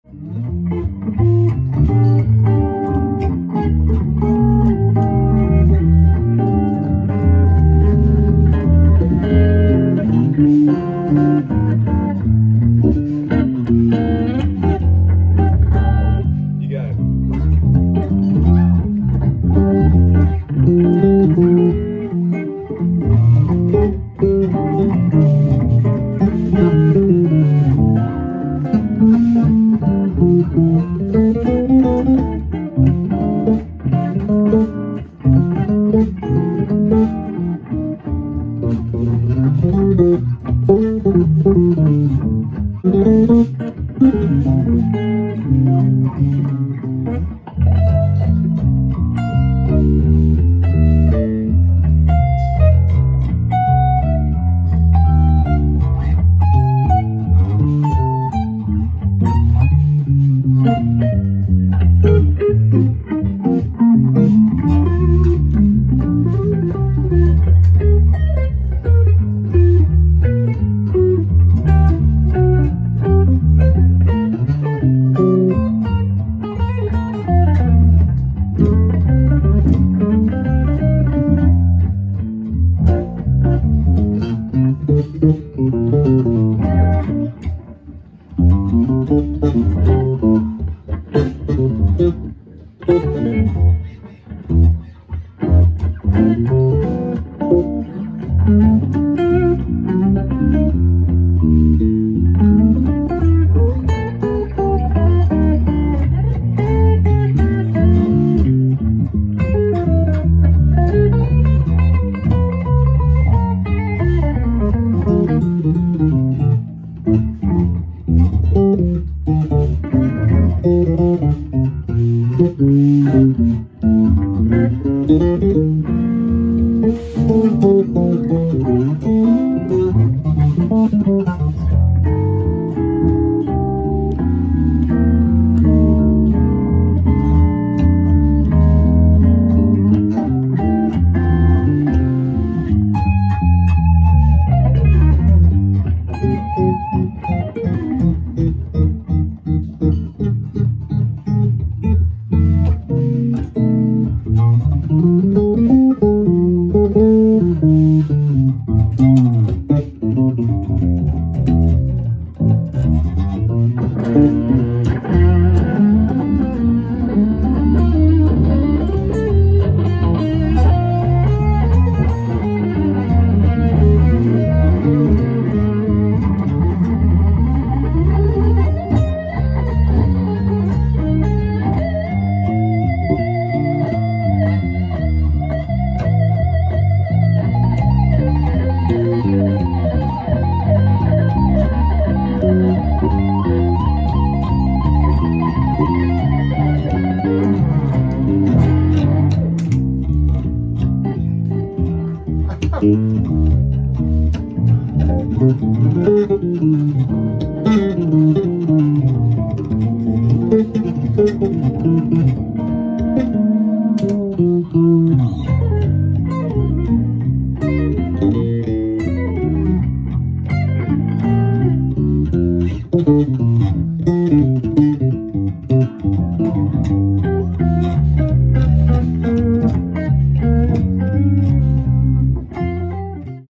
Miami Recording Studio Blues Jam at Blue Road Records Studio, Miami, Florida
An impromptu blues jam breaks out with 2 members of the Blue Road Records Studio Sessions Band in between recording session takes!  Brev Sullivan on guitar, Javier Espinoza on bass.  Filmed at Blue Road Records Studio in beautiful Miami, Florida.